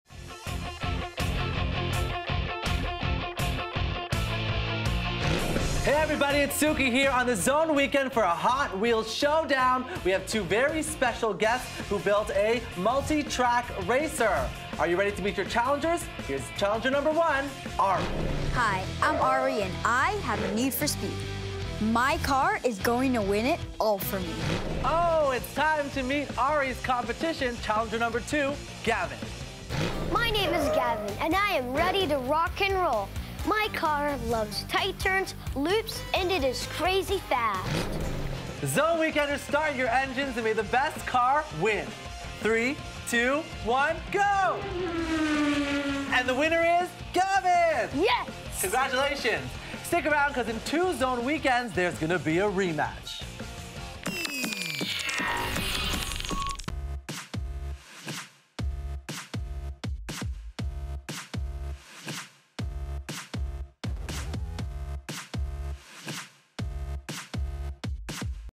Hot Wheels Showdown | The Zone Weekend
Watch special guests Ari and Gavin have an epic showdown with the Hot Wheels multi-track racer!